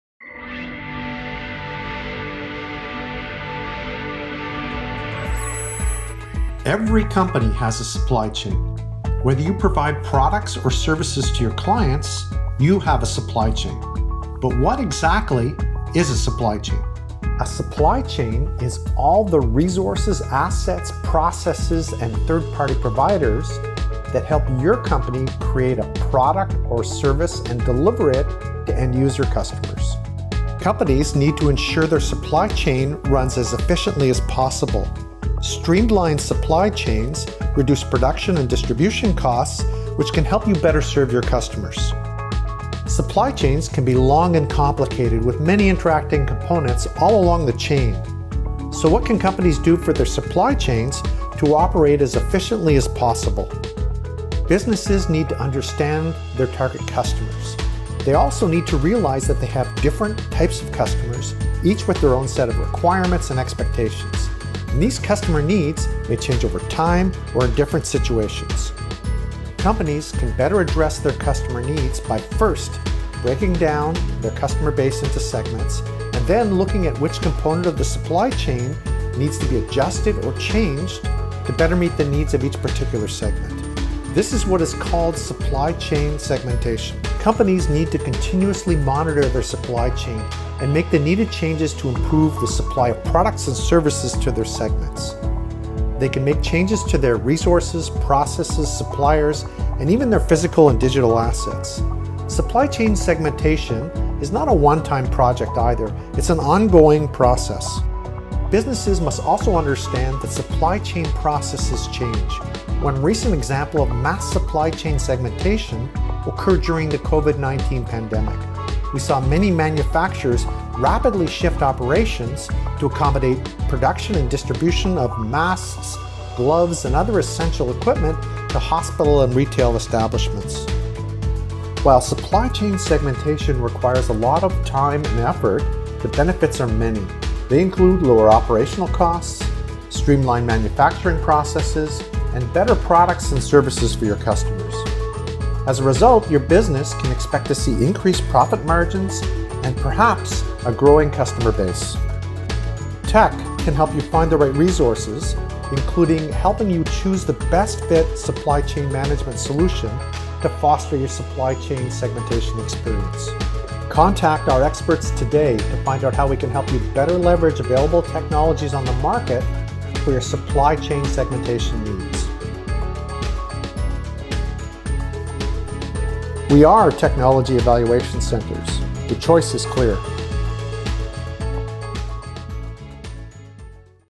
What is Supply Chain Segmentation? | Supply Chain Management
Businesses across the globe rely on various resources to create their products and get them into the hands of waiting customers. Those tools, materials, and other assets make up what’s known as an organization’s supply chain. Ideally, supply chains enable businesses to reduce their production and logistics costs while aiding them in distributing their products more efficiently and effectively.

In this video, the TEC team details how companies can implement supply chain segmentation strategies to remain competitive and better align their supply chains with their overall goals.

Chapters:

00:00 What is Supply Chain?
00:51 Tips to Manage Supply Chains
02:14 Benefits of Supply Chain Segmentation